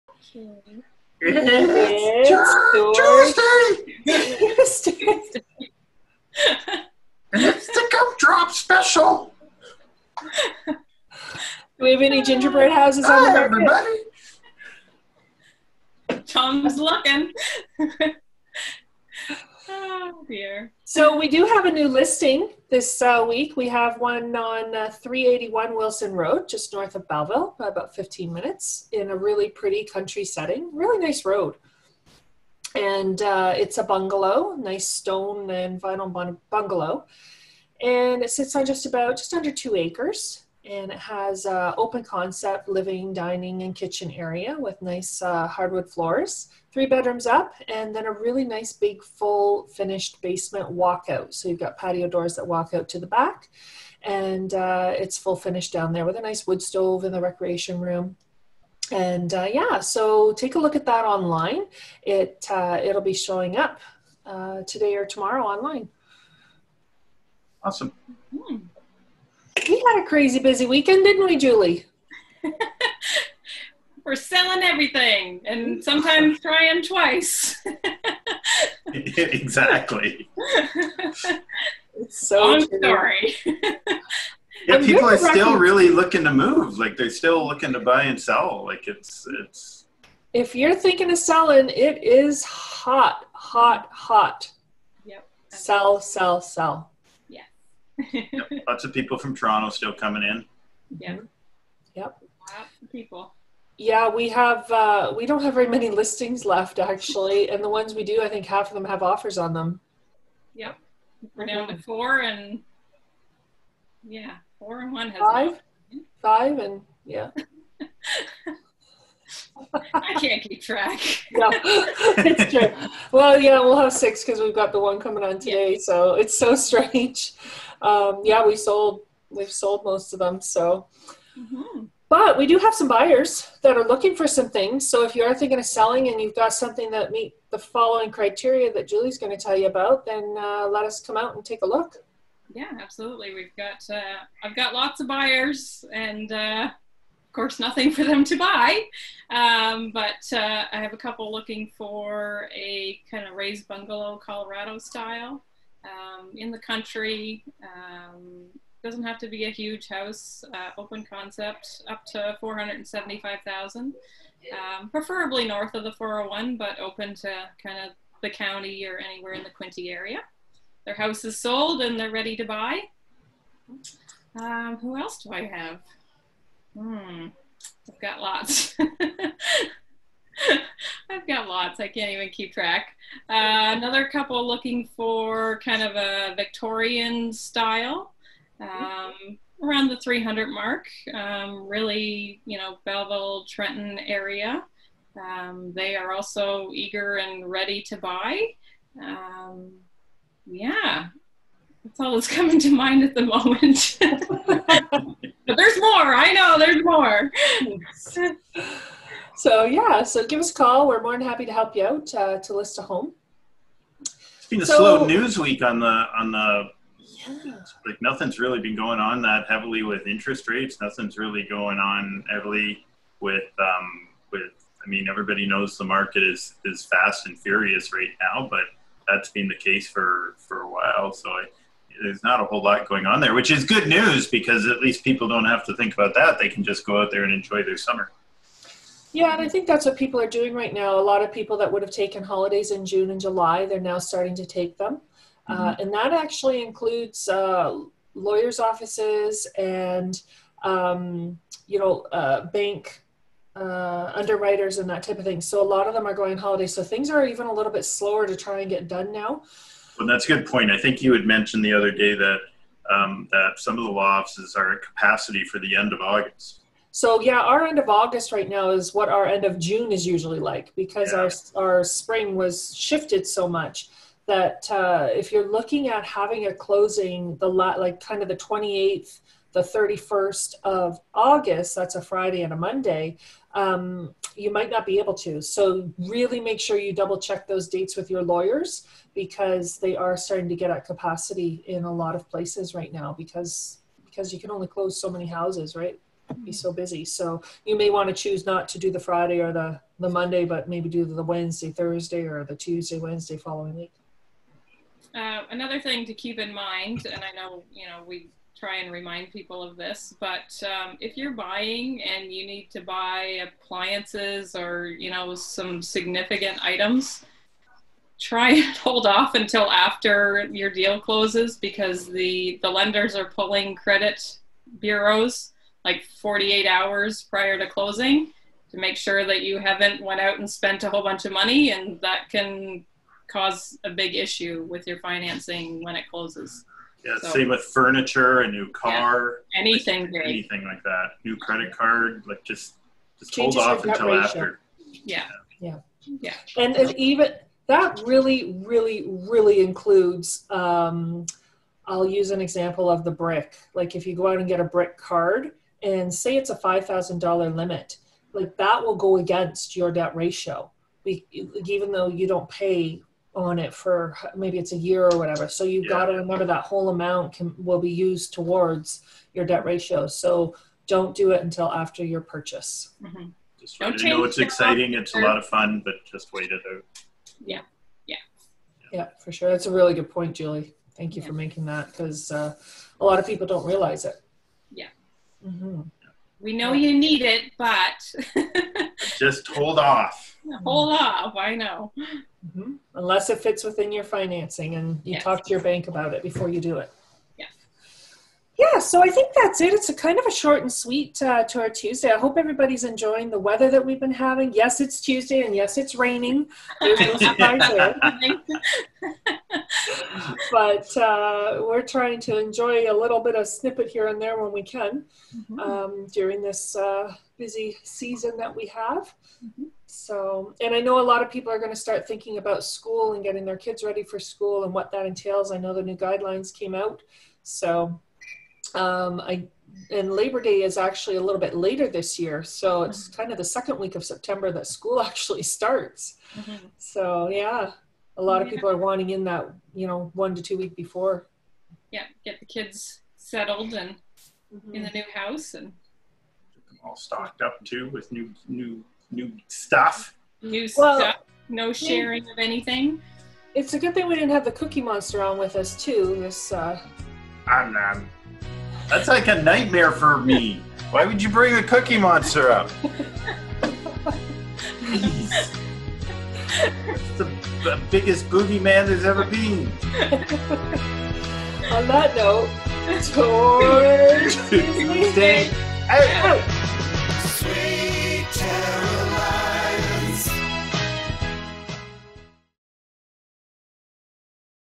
Tour Tuesday and the Ginger Bread Man
[00:00:04.790] - Team Tara Lyons
It's Tour Tuesday!

[00:00:04.850] - Tom Lyons
It's the gumdrop special.. Hi everybody!

[00:00:16.560] - Tara Lyons
 So we do have a new listing this week, we have one on 381  Wilson Road, just north of Bellville for about 15 minutes, in a really pretty country setting. Really nice road. And it's a bungalow, a nice stone and fine bungalow, and it sits on just about just under two acres and it has open concept living, dining and kitchen area with nice hardwood floors, three bedrooms up, and then a really nice big full finished basement walk out. So you've got patio doors that walk out to the back and it's full finished down there with a nice wood stove in the recreation room. And yeah. So take a look at that online. It it'll be showing up today or tomorrow online.

[00:01:24.700] - Tom Lyons
People are still really looking to move like they're still looking to buy and sell like it's..

[00:01:30.130] - Tara Lyons
If you're thinking of selling, it is hot, hot, hot.. Sell, sell, sell..

[00:01:38.560] - Tom Lyons
Yup! Lots of people from Toronto still coming in.

[00:01:40.970] - Team Tara Lyons
Yup, yeah. Lots of people.

[00:01:44.980] - Tara Lyons
We don't have very many listings left, actually. And the ones we do, I think half of them have offers on them.

[00:02:02.980] - Tara Lyons
Well, you know six, because we've got the one coming on today. So it's so strange.. Yeah, we sold, we've sold most of them. So, but we do have some buyers that are looking for some things. So if you are thinking of selling and you've got something that meet the following criteria that Julie's going to tell you about, then let us come out and take a look.

[00:02:22.360] - Julie Martin
Yeah, absolutely! We've got.. I've got lots of buyers and of course, nothing for them to buy.. But I have a couple looking for a kind of raised bungalow Colorado style in the country, doesn't have to be a huge house, open concept, up to $475,000, preferably North of the 401, but open to kind of the county or anywhere in the Quinte area..

[00:03:34.920] - Tom Lyons
Been a slow newsweek on the..  Nothing's really been going on that heavily with interest rates. Nothing's really going on heavily with uhm.. I mean everybody knows the market is fast and furious right now, but that's been the case for for a while. So it's not a whole lot going on there, which is good news because at least people don't have to think about that. They can just go out there and enjoy their summer.

[00:03:59.110] - Tara Lyons
Yeah, I think that's what people are doing right now. A lot of people that would have taken holidays in June and July, they're now starting to take them. And that actually includes lawyer's offices and, you know, bank underwriters and that type of thing. So a lot of them are going on holiday. So things are even a little bit slower to try and get  done now.

[00:04:26.770] - Tara Lyons
So, yeah, our end of August right now is what our end of June is usually like because  our Spring was shifted so much, that if you're looking at having a closing.. the lot like kind of the 28th, the 31st of August, that's a Friday and a Monday, you might not be able to..

[00:05:05.540] - Julie Martin
Another thing to keep in mind, and I know we try and remind people of this, but if you're buying and you need to buy appliances or you know, some significant items, try to hold off until after your deal closes because the lenders are pulling credit bureaus like 48 hours prior to closing, to make sure that you haven't went out and spent a whole bunch of money and that can cause a big issue with your financing when it closes.

[00:05:47.500] - Tara Lyons
That really, really, really includes.. I'll use an example of the brick. Like if you go out and get a brick card and say it's a $5000 dollar limit, that will go against your debt ratio, even though you don't pay on it for maybe it's a year or whatever. So you've got to remember that whole amount will be used towards your debt ratio. So don't do it until after your purchase.

[00:06:17.260] - Tara Lyons
Yup! For sure it's a really good point, Julie! Thank you for making that because a lot of people don't realize it.

[00:06:38.380] - Tara Lyons
 Yeah. So I think that's it. It's a kind of a short and sweet Tour Tuesday. I hope everybody's enjoying the weather that we've been having. Yes, it's Tuesday and yes, it's raining..

[00:07:33.970] - Tara Lyons
It's a good thing we didn't have the Cookie Monster on with us to so..

[00:07:38.760] - Tom Lyons
That's like a nightmare for me. Why would you bring a Cookie Monster out? The biggest boogeyman there's ever been.